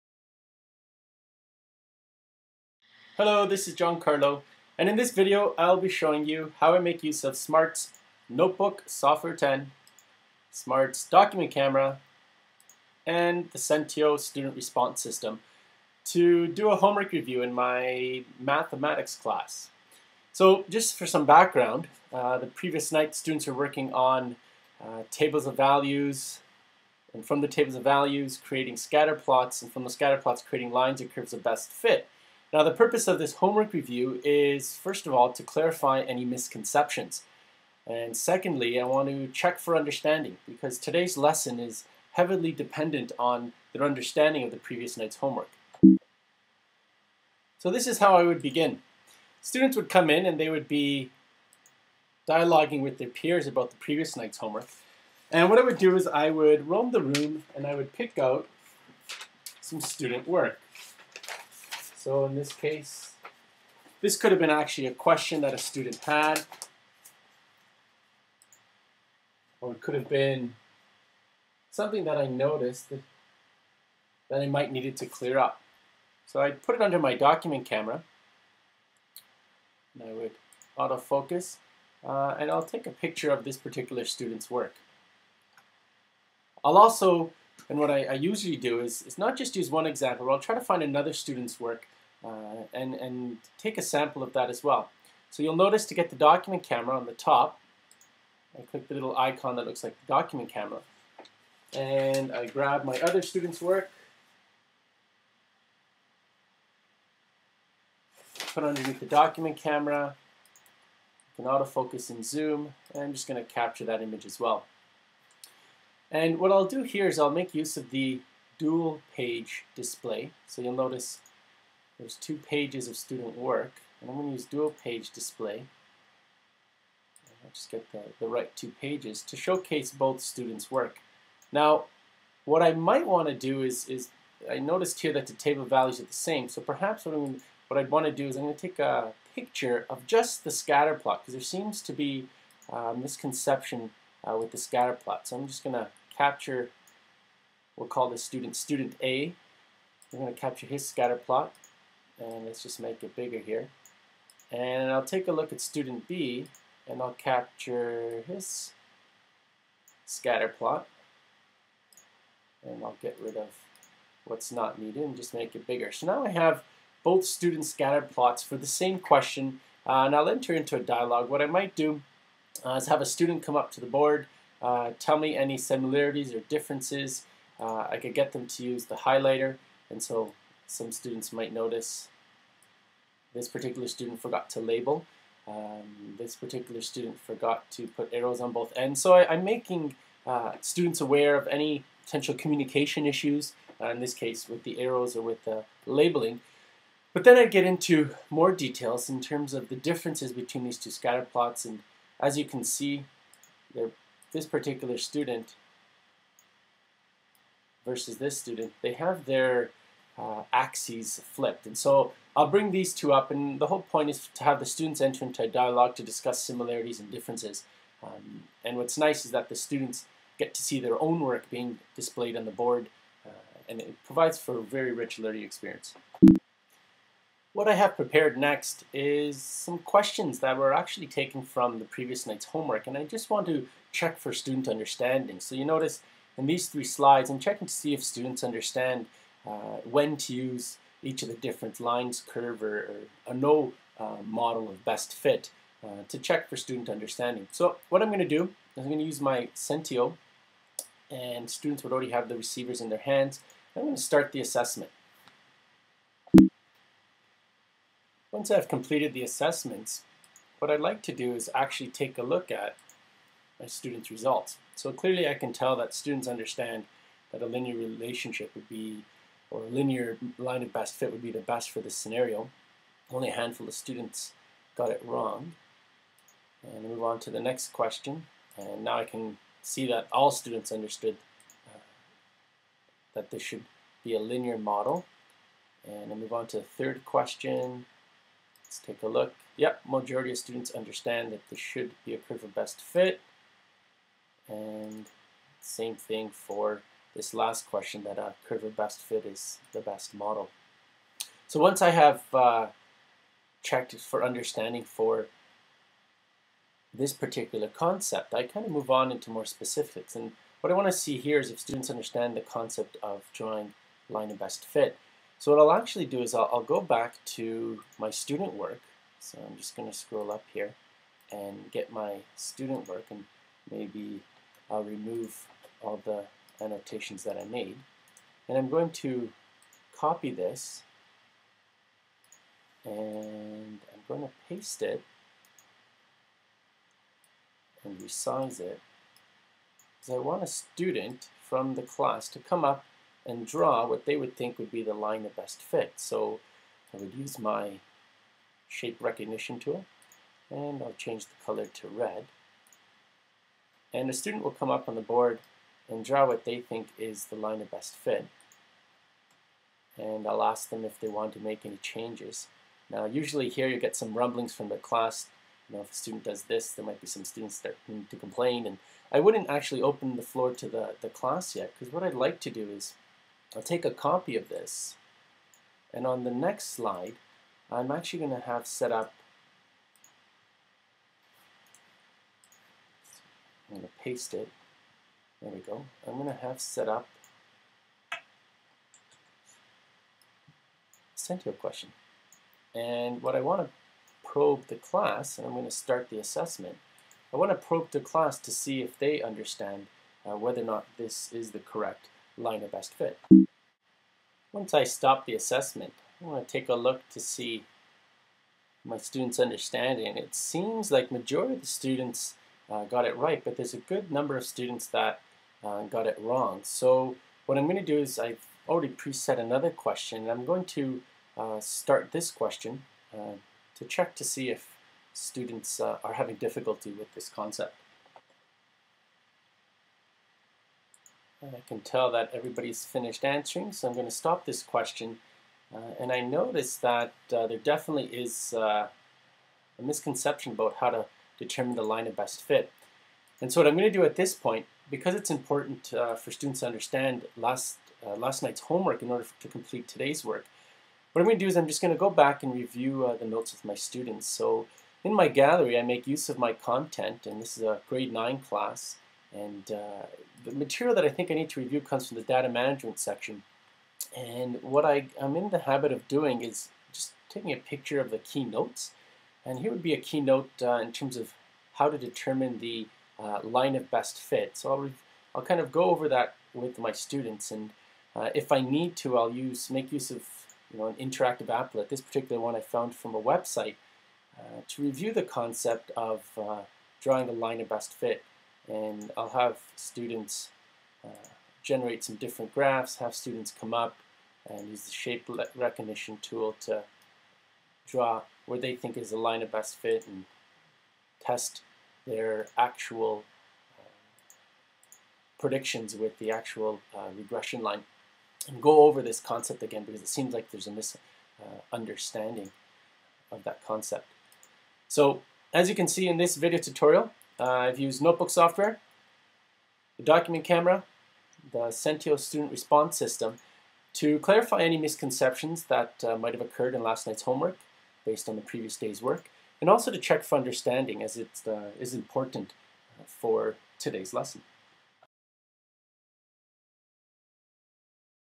Math Homework Takeup
Watch SMART's Education Consultant in Ontario show you how he uses Notebook10 software, the SMART Document Camera, and the SENTEO student response system, to take up homework before the beginning of a new lesson.  The content of this video is taken from a grade 9 academic Ontario mathematics class. Students would have completed the units on Algebra, and Data Management and are beginning the Linear Relations Unit.